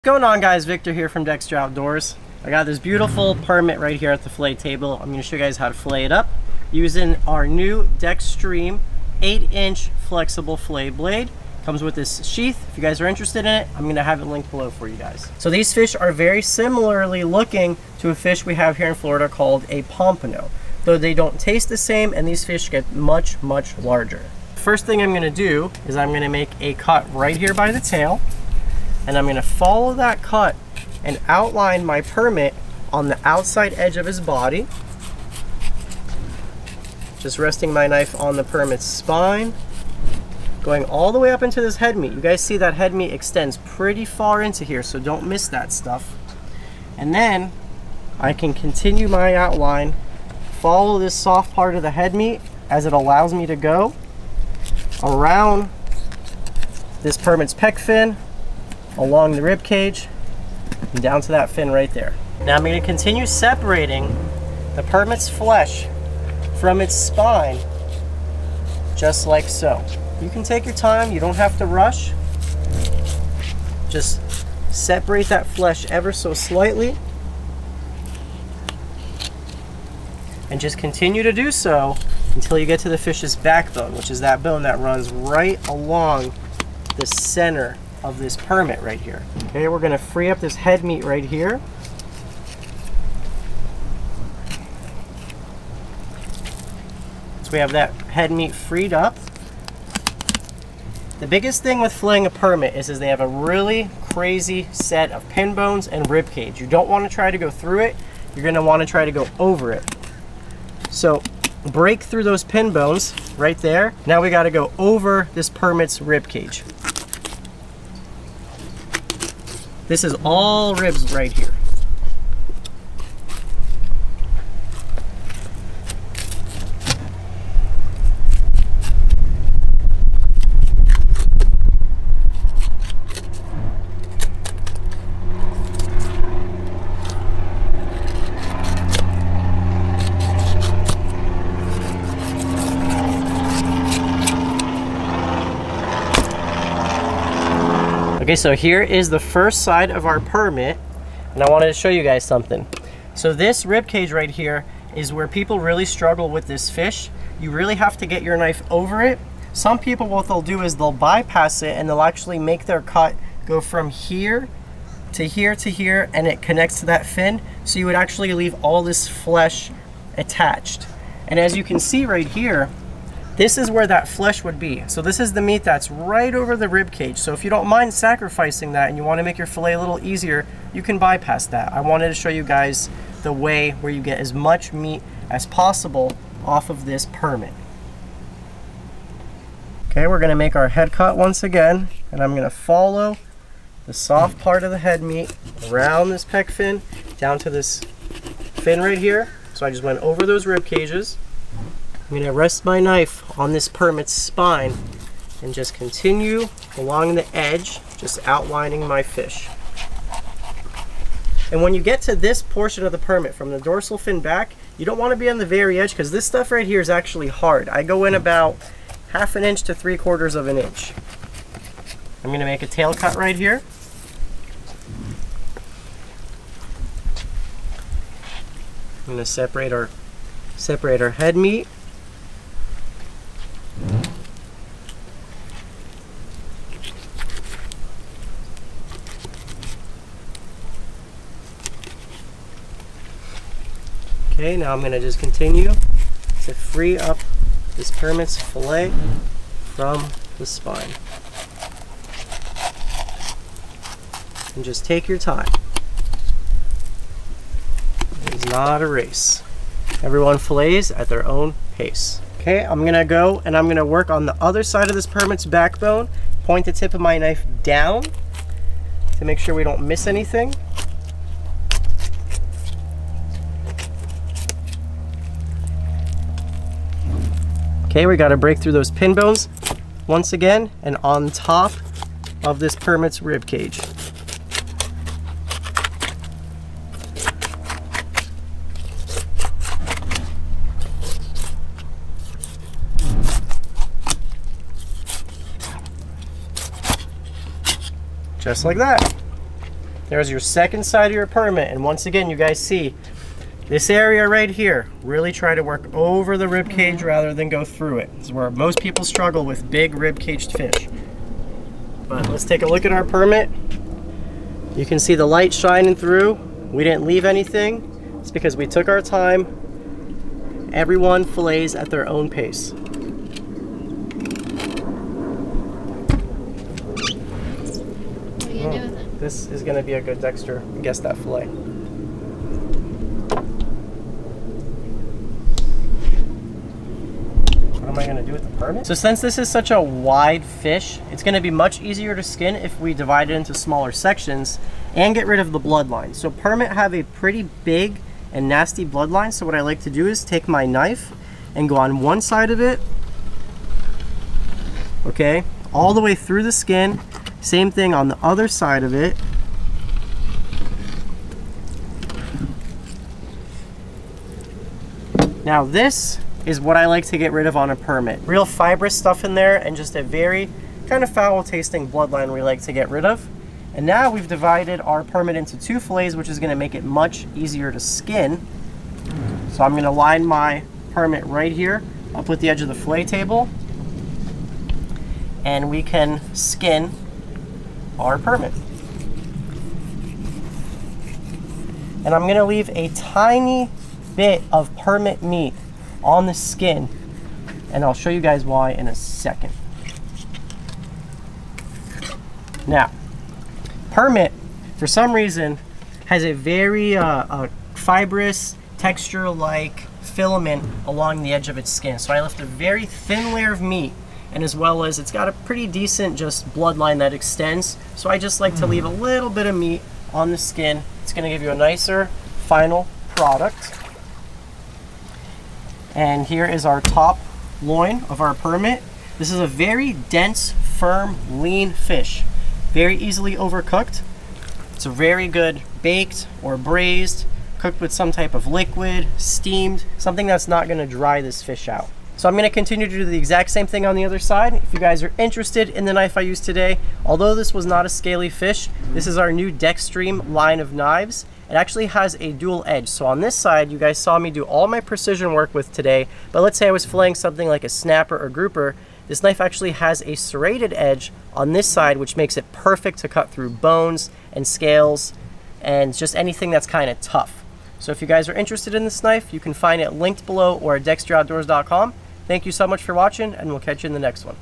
GIANT Permit! How to Fillet Florida Permit!
In this video we show you how to fillet a giant Florida permit. These fish can be tricky to clean, but watch this video to learn some fillet tips and tricks to make your job easy. Permit are a firm and lean fish, that can easily be overcooked. They are best either fried, eaten as sashimi, ceviche or cooked with liquid to avoid drying out.